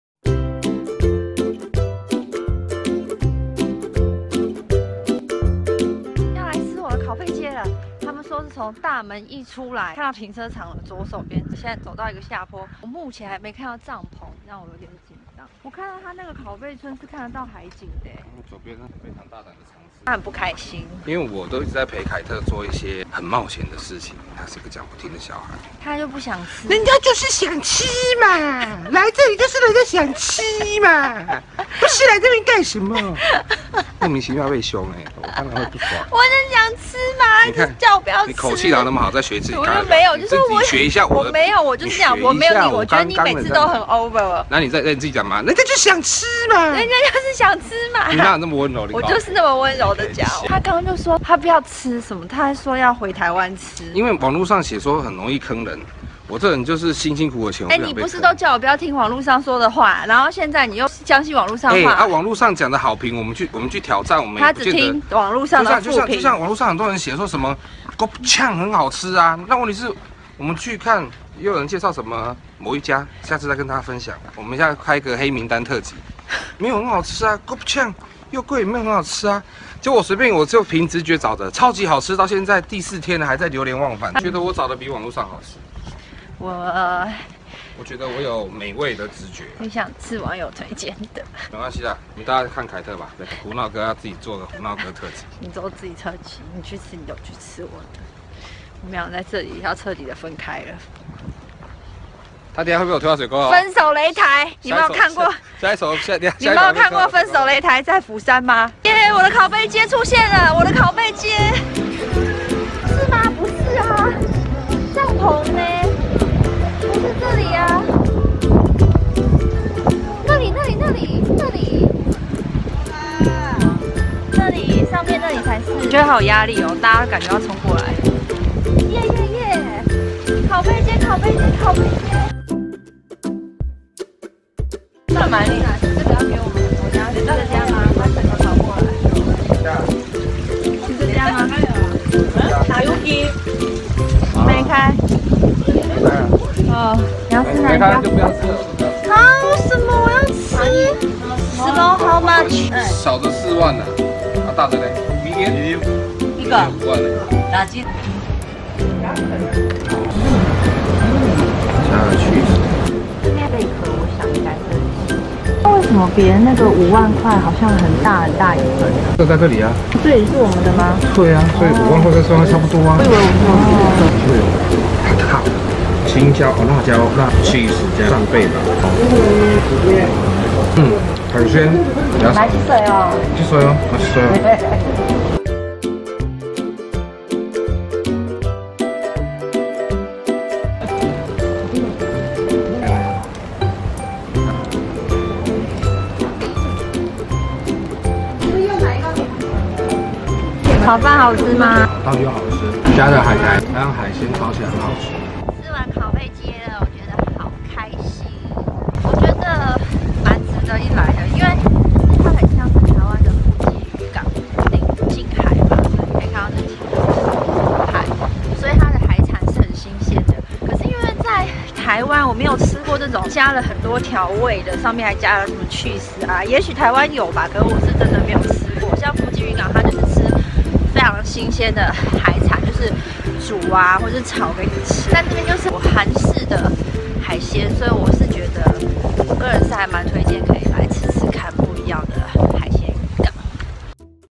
【釜山美食】太宗台烤貝街/ 釜山旅遊/ 釜山自由行
♥ 你訂閱凱特愛跟瘋了嗎?

來釜山旅遊就是要吃海鮮阿!!
太宗台烤貝街
可以吃烤貝搭配著炒飯真是太完美

這趟釜山行去太宗台看完美景
再去烤貝街吃烤貝，完全滿足❤❤❤❤

想看更多凱特釜山旅遊VLOG